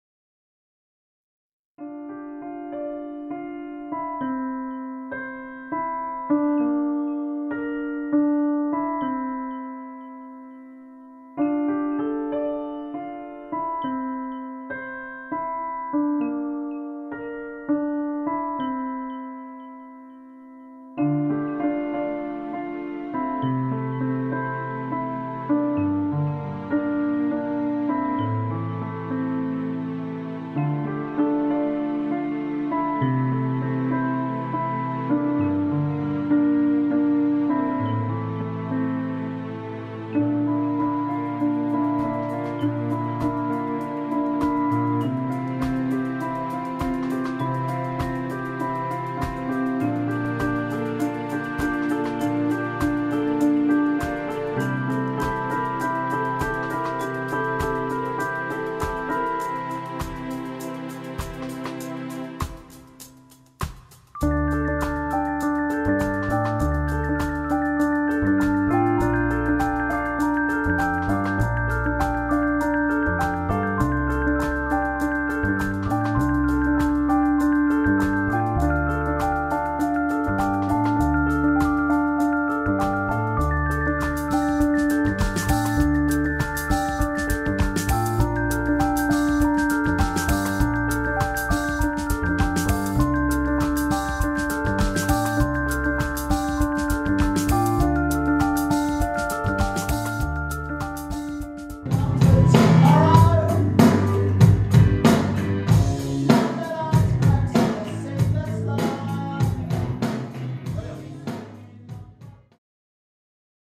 Auditions janvier 2014
Compilation des prestations des élèves de musique et chant du centre Victor Gelez.